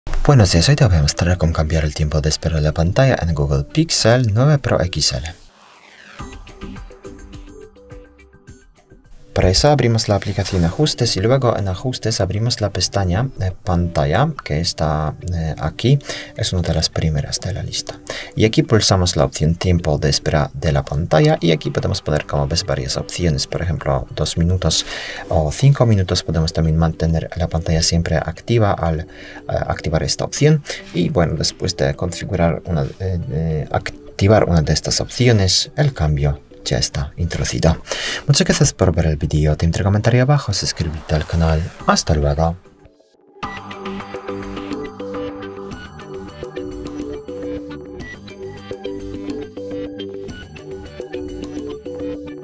Cómo Cambiar el Tiempo de Espera de la Pantalla en el Google Pixel 9 Pro XL
En este video, te mostramos cómo ajustar el tiempo de espera de la pantalla en tu Google Pixel 9 Pro XL. Aprende a configurar cuánto tiempo debe pasar antes de que la pantalla se apague automáticamente para ahorrar batería o personalizar la duración según tus preferencias.

¿Dónde se encuentra la opción para ajustar el tiempo de espera de la pantalla en el Pixel 9 Pro?
¿Cuánto tiempo de espera es recomendable para conservar la batería?
¿Cómo ajustar el tiempo de pantalla para diferentes aplicaciones en el Pixel 9 Pro XL?